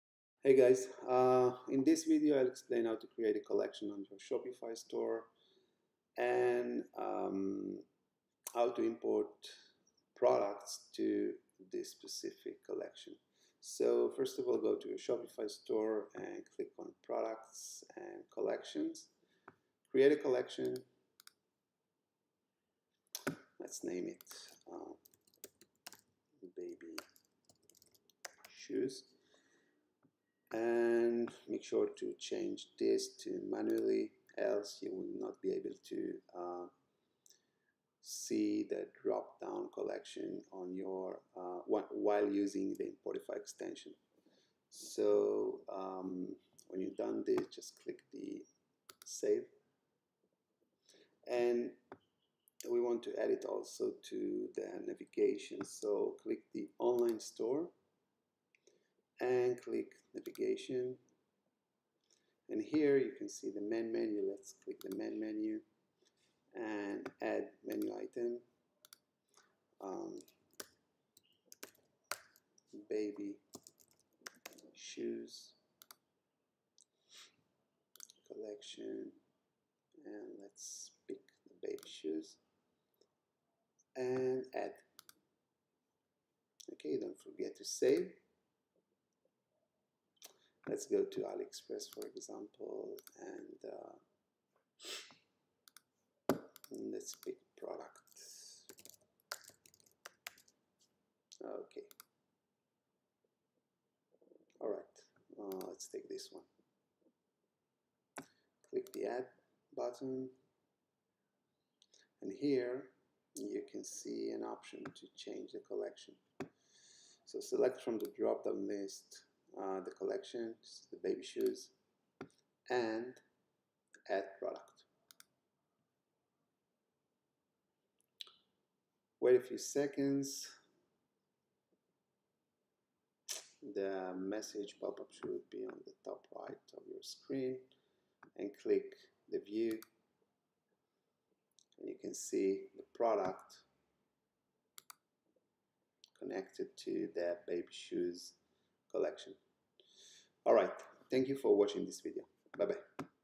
How to import product to specific collection using Importify
Working with a few collections on Shopify can be confusing, check out this video below to learn how to set it up.